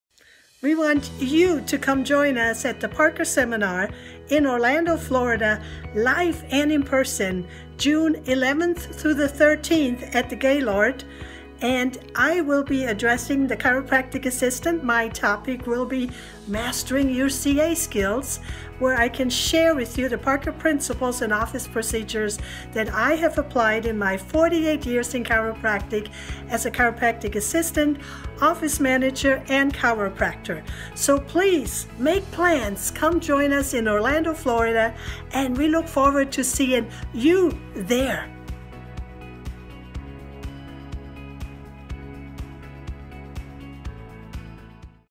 Rose Lepien - Parker Orlando 2021
Dr. Rose Lepien, a native of Germany, is the owner and Senior Doctor at Aaragon Chiropractic Clinic in Lawton, Oklahoma with more than 48 years of experience applying the Parker Principles, Philosophies and Practice management strategies on a daily basis in her practice. She offers a wealth of experience and knowledge relating to issues of employment, staff management, associate Doctors, patient treatment and case management as well as marketing and advertising.

She is a 1991 Graduate of Parker University. She has journeyed the path as a chiropractic assistant, office manager, student and Doctor of Chiropractic. She was a wife of a Chiropractor and mother of three and grandmother of two Parker graduates and successful Doctor for 30 years. Dr. Lepien has been very active in leadership roles and speaking engagements with Parker University, on the Board of Trustees, Parkers Seminars, WCWC, LCW, and in her local community. She has published and co-written several books.